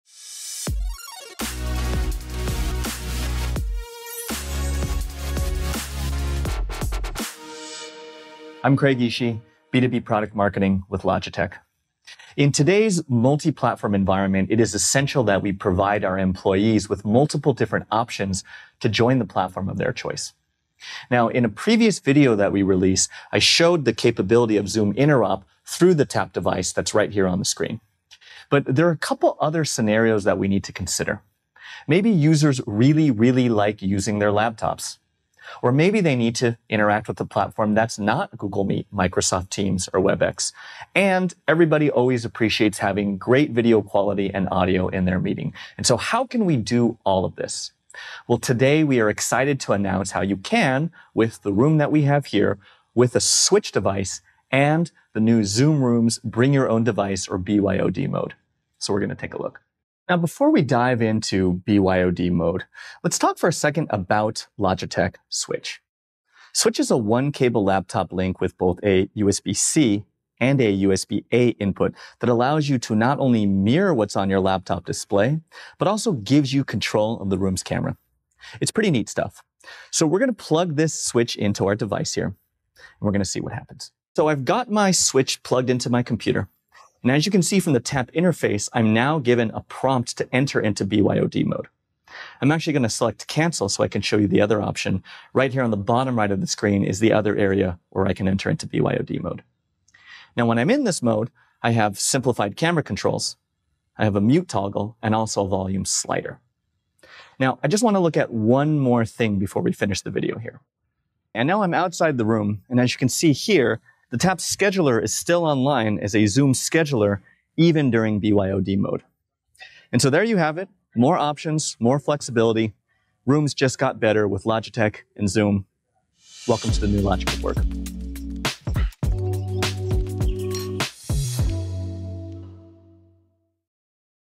Zoom BYOD Mode and Logitech
Introducing Zoom Rooms' Bring Your Own Device (BYOD) Mode, designed to enhance collaboration in today's multi-platform environment. This interface offers meeting controls while using the camera in BYOD mode and keeps devices like Tap Scheduler online.